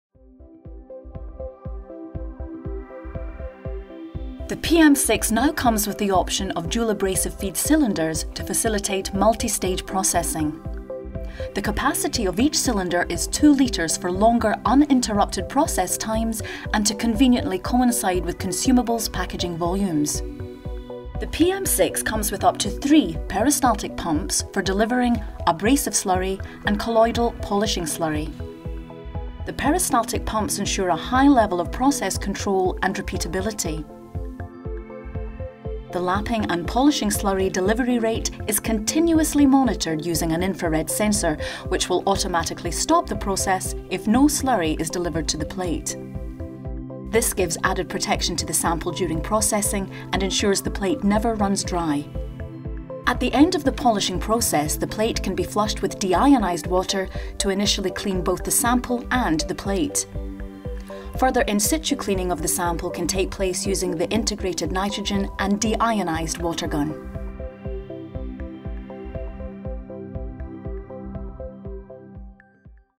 Logitech Ltd. PM6 - Metered Abrasive Delivery
Logitech Ltd. PM6 Precision Lapping & Polishing System - With dual metered abrasive cylinders with 2L capacity